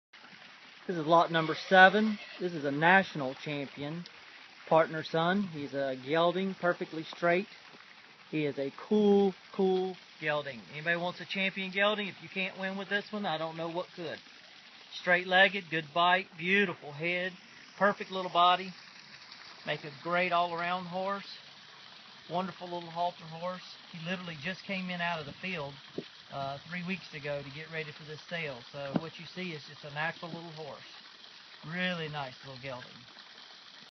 Aloha Select Sale LOT #7 National Champion! Bid November 8th!
Lot# 7 : Aloha Acres A Silent Situation
DOB: 5/12/2013
AMHA: IN OFFICE
AMHR: 323342
ASPC: N/A
Gender: Geldings
Color: Sorrel Pinto
Height: Mature
AMHR NATIONAL CHAMPION GELDING as a weanling!

2013 Chestnut Pinto yearling gelding - He's already a National Champion - Now this stellar gelding will make the ultimate all around! Pretty movement, great attitude - can win in halter, color and performance! He's fit and ready to go home with you and win!